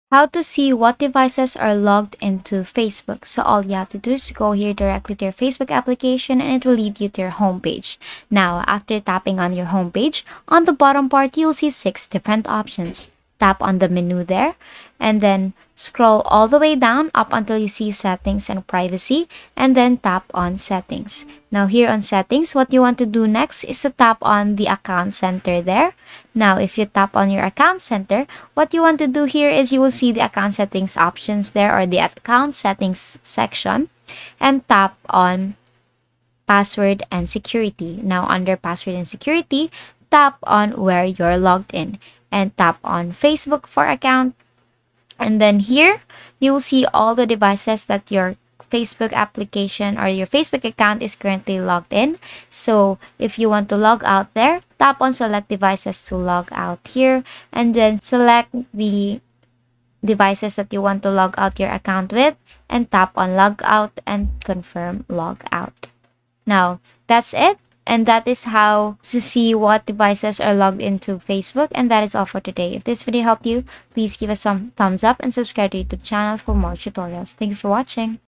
How To See What Devices are Logged into Facebook (2023)
Learn‎‎‏‏‎ ‎How To See What Devices are Logged into Facebook

In this video I will show you‏‏‎ ‎how to see what devices are logged into facebook